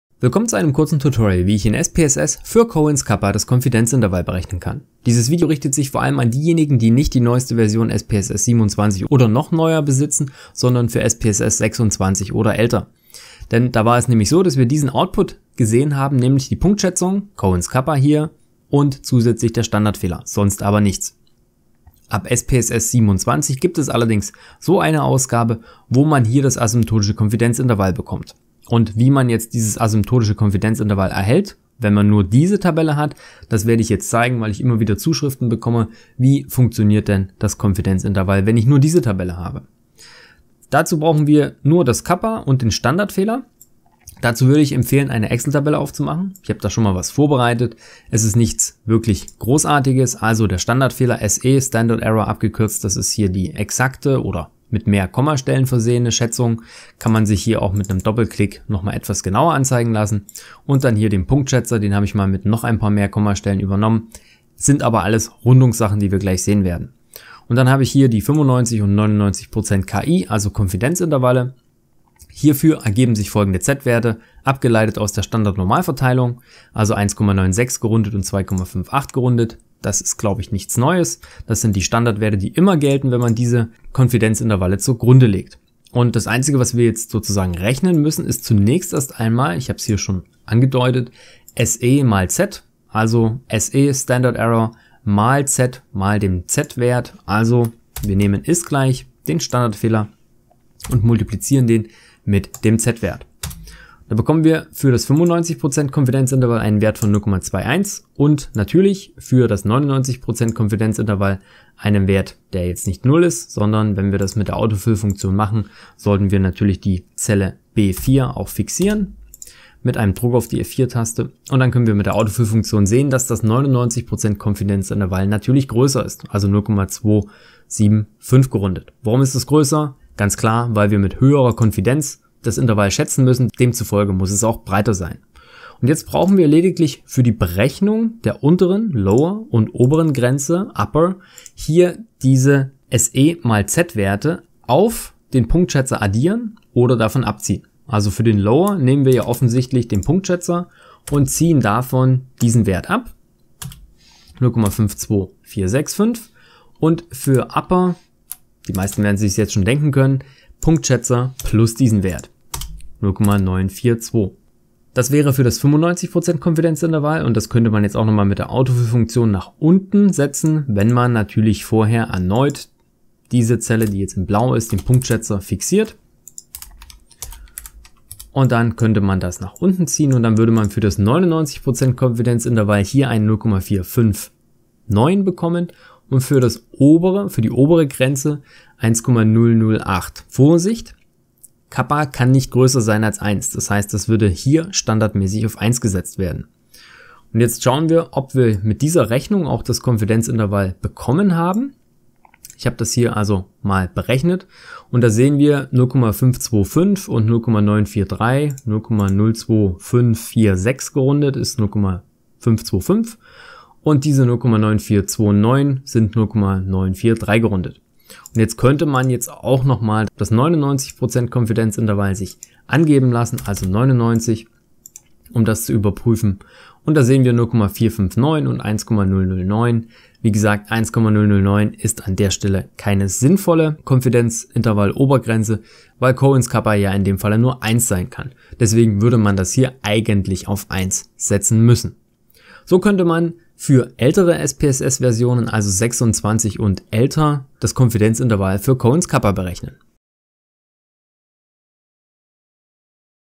Konfidenzintervalle für Cohens Kappa in SPSS berechnen (SPSS 26 und älter)
// Konfidenzintervalle für Cohens Kappa in SPSS berechnen //

In SPSS-Versionen 26 und älter wird kein 95% oder 99%-Konfidenzintervall für Cohens Kappa ausgegeben. In diesem Video zeige ich, wie man mithilfe des Standardfehlers das Konfidenzintervall nachträglich berechnet.

Hinweis: In SPSS Versionen 27 und neuer wird das Konfidenzintervall bereits automatisch ausgegeben.

Bei Fragen und Anregungen zu Konfidenzintervalle für Cohens Kappa in SPSS berechnen, nutzt bitte die Kommentarfunktion.

Zeitstempel ⏰
0:00 Einleitung und Ausgangssituation
0:40 Kappa und Standardfehler in Excel kopieren
1:25 Schwankungswert mit z-Wert berechnen
2:15 Obere und untere Konfidenzintervallgrenze berechnen